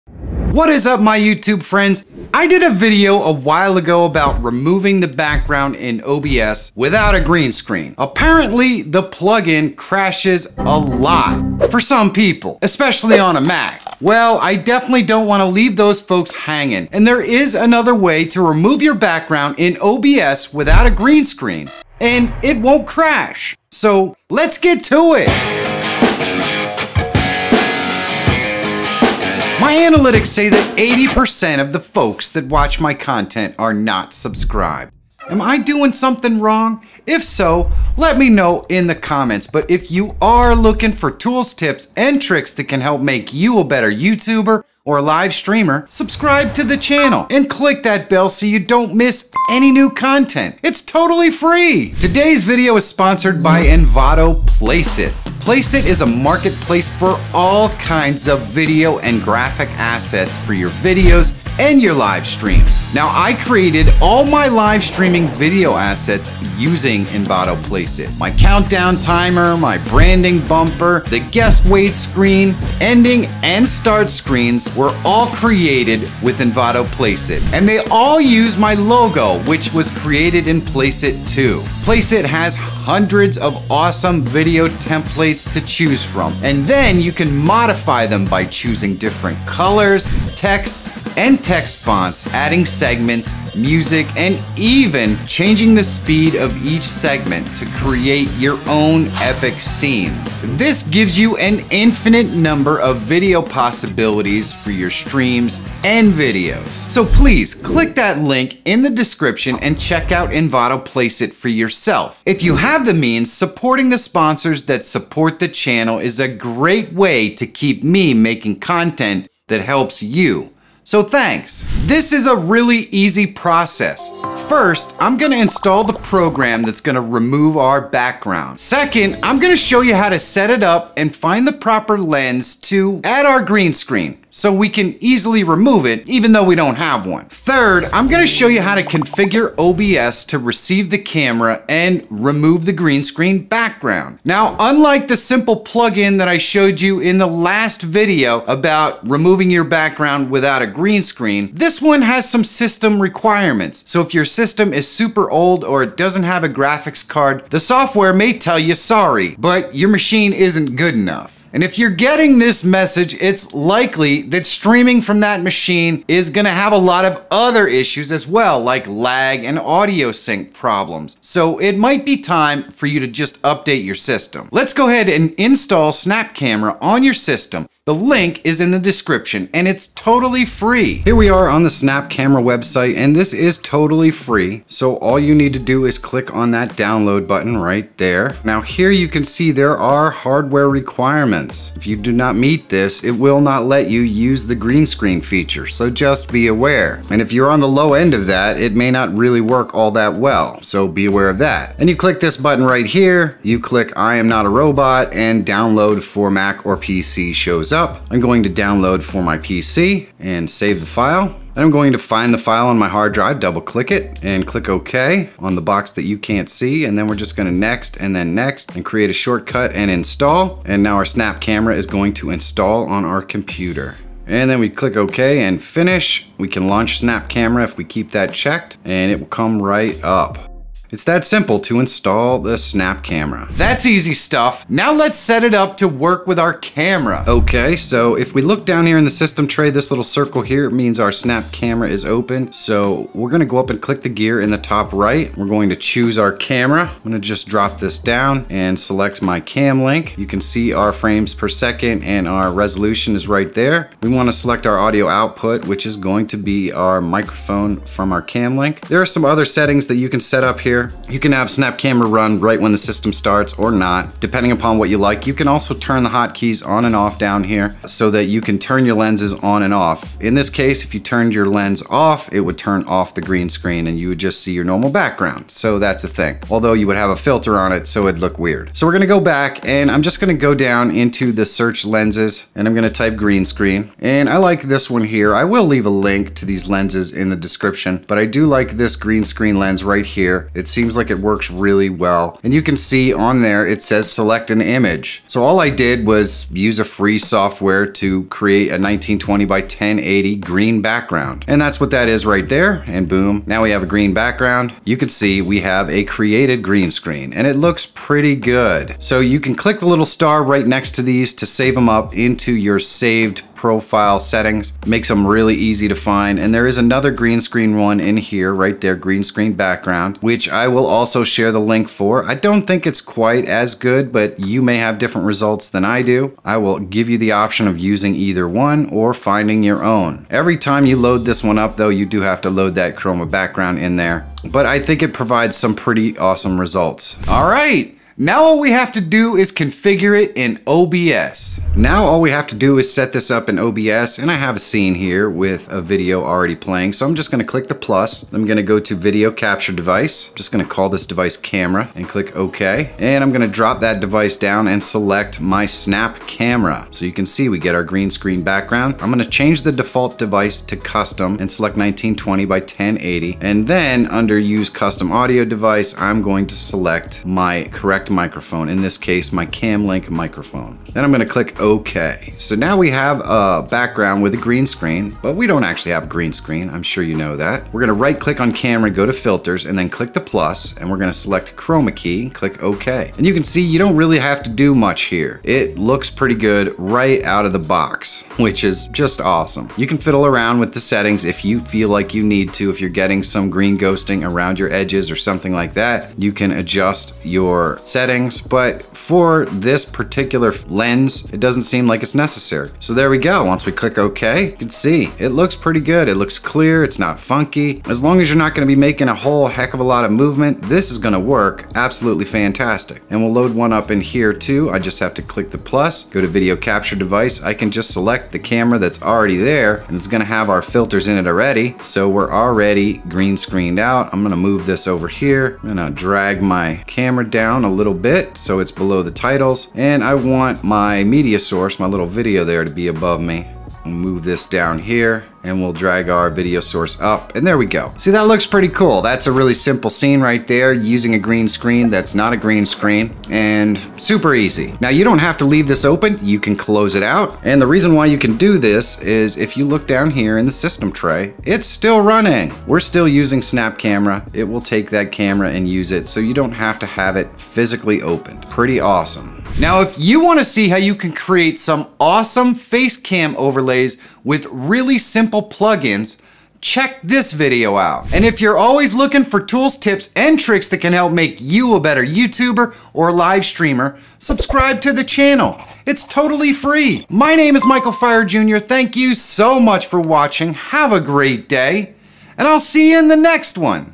OBS Remove Your Background Without a Green Screen - No Crashes - MAC & StreamLabs OBS
You can remove your background in OBS, StreamLabs OBS, and other streaming software Without a green screen even on a Mac. With no crashing!Today I show you how!